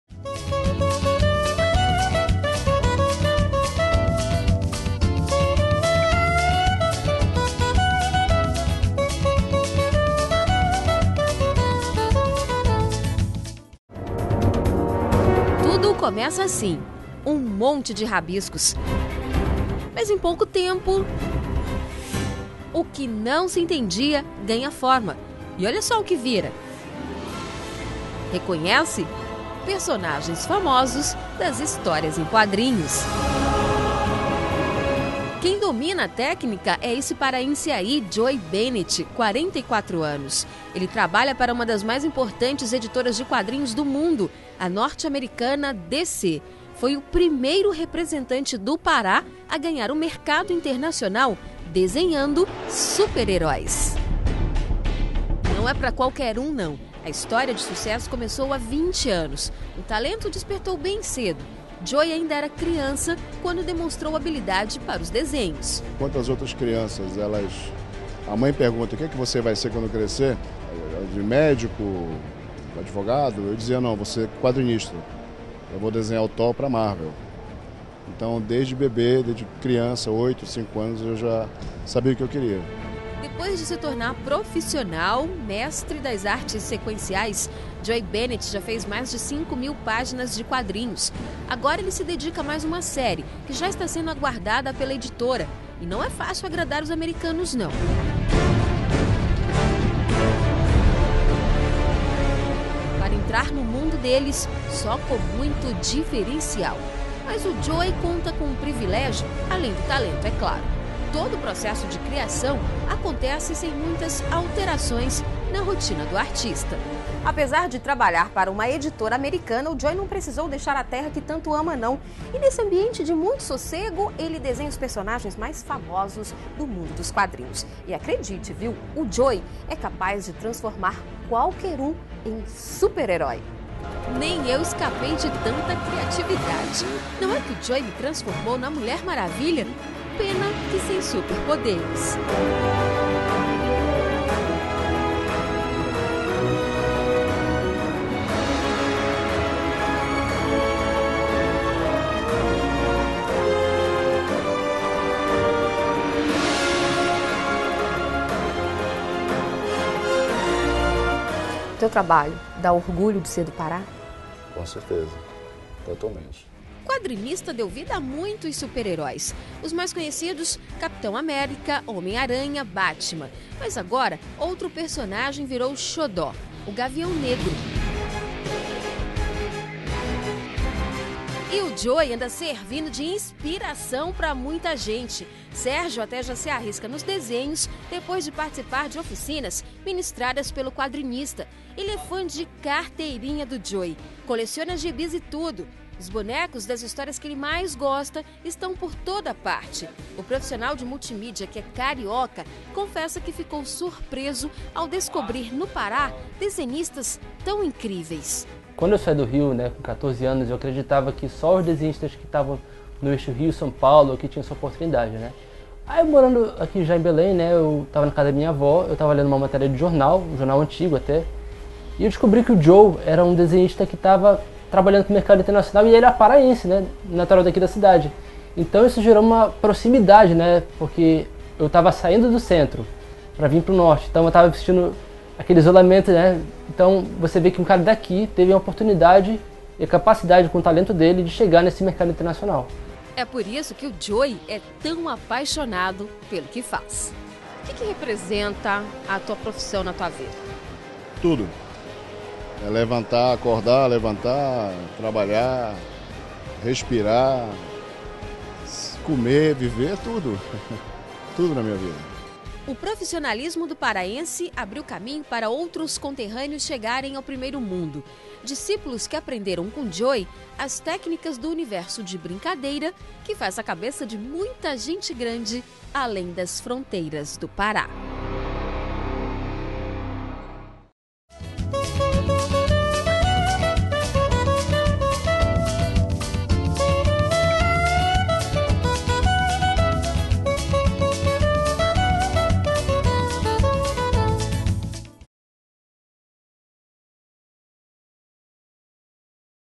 Orgulho do Pará: paraense está entre os melhores quadrinistas do mundo
Assista a reportagem da RBA TV